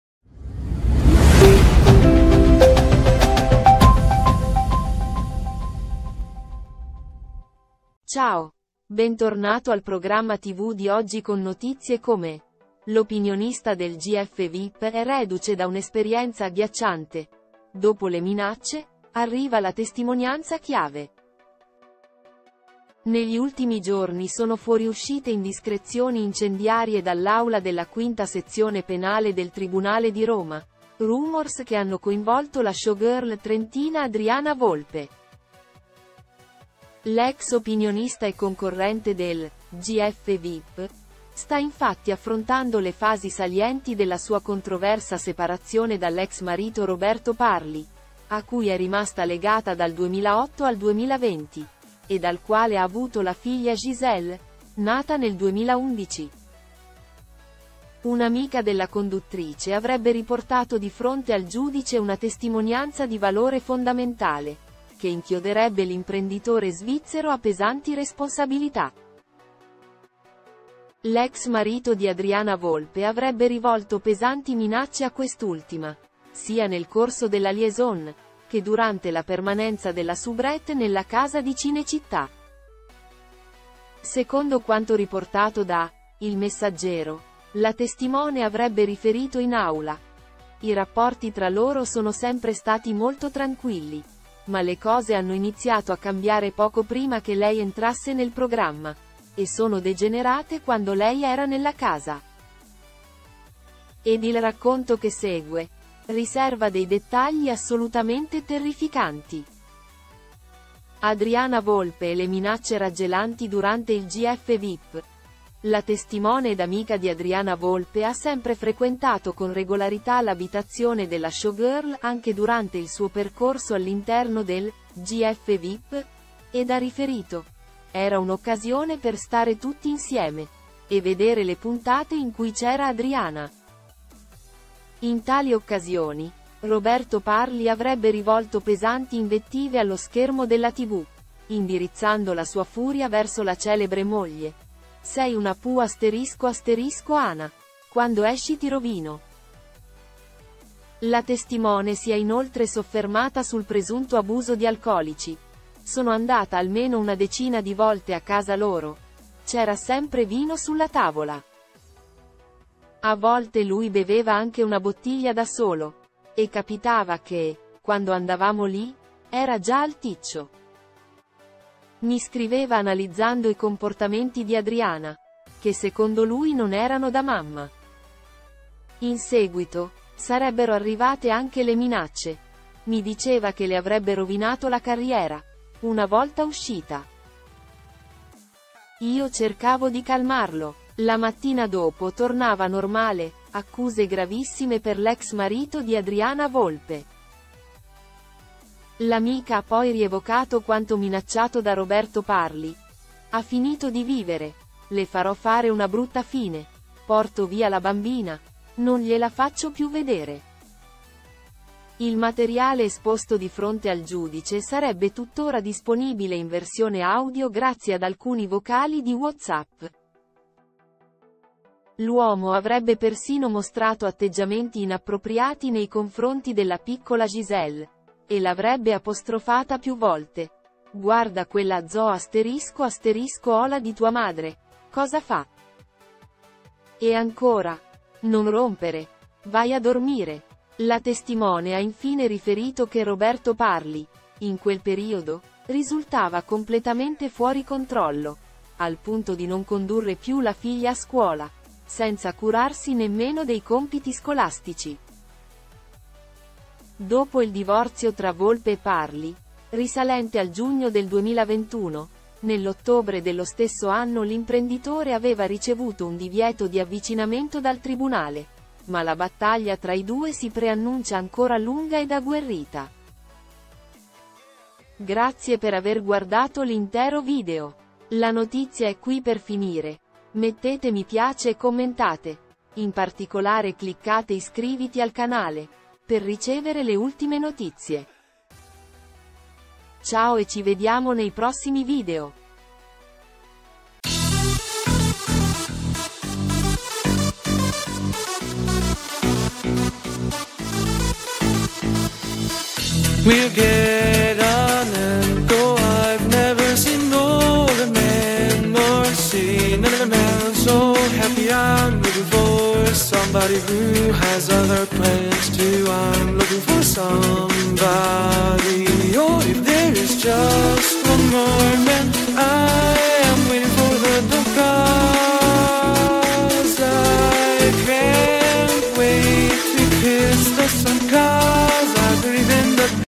Opinionista del GF Vip vittima di violenza “Farà una brutta fine”
Negli ultimi giorni sono fuoriuscite indiscrezioni incendiarie dall’aula della quinta sezione penale del Tribunale di Roma, rumors che hanno coinvolto la showgirl trentina Adriana Volpe.

L’ex opinionista e concorrente del “GF Vip” sta infatti affrontando le fasi salienti della sua controversa separazione dall’ex marito Roberto Parli, a cui è rimasta legata dal 2008 al 2020, e dal quale ha avuto la figlia Gisele, nata nel 2011.

Un’amica della conduttrice avrebbe riportato di fronte al giudice una testimonianza di valore fondamentale, che inchioderebbe l’imprenditore svizzero a pesanti responsabilità. L’ex marito di Adriana Volpe avrebbe rivolto pesanti minacce a quest’ultima, sia nel corso della liaison, che durante la permanenza della soubrette nella casa di Cinecittà.